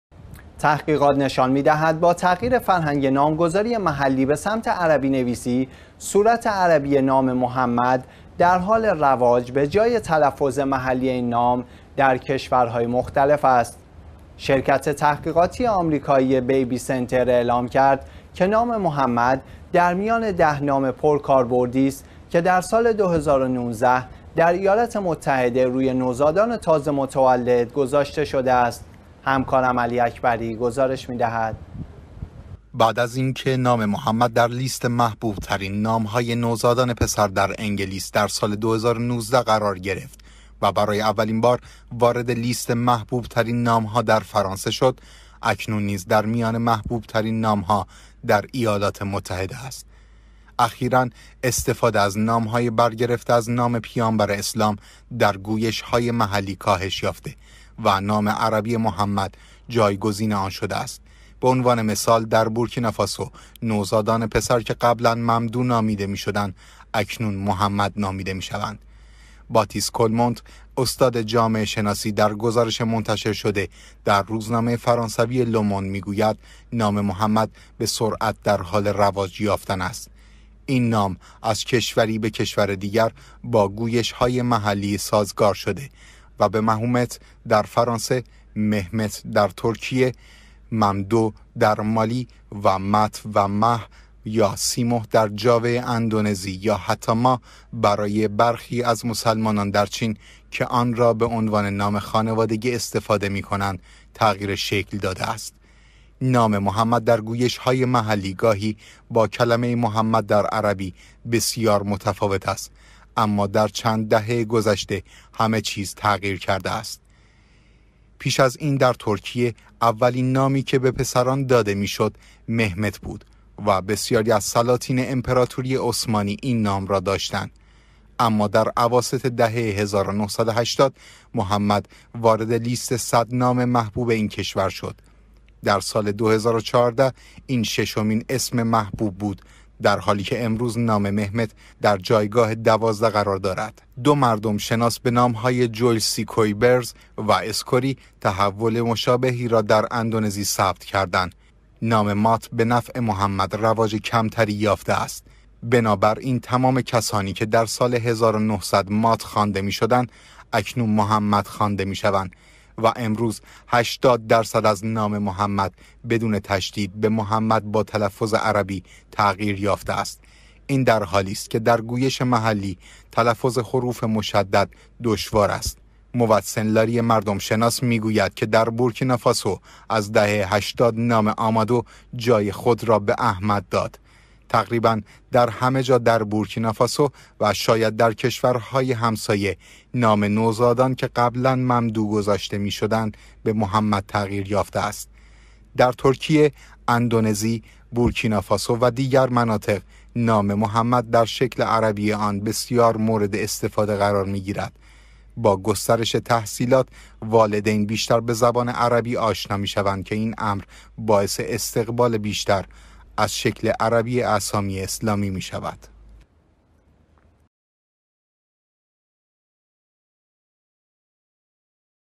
محبوبیت نام محمد در کشورهای جهان
تحقیقات نشان می‌دهد با تغییر فرهنگ‌ نامگذاری محلی به سمت عربی‌نویسی، صورت عربی نام «محمد» در حال رواج به جای تلفظ محلی این نام در کشورهای مختلف است.
شرکت تحقیقاتی آمریکایی «Baby Center» اعلام کرد که نام «محمد» در میان ۱۰ نام پرکاربردی است که در سال ۲۰۱۹ در ایالات متحده روی نوزادان تازه متولد گذاشته شده است.

علی اکبری گزارش می دهد: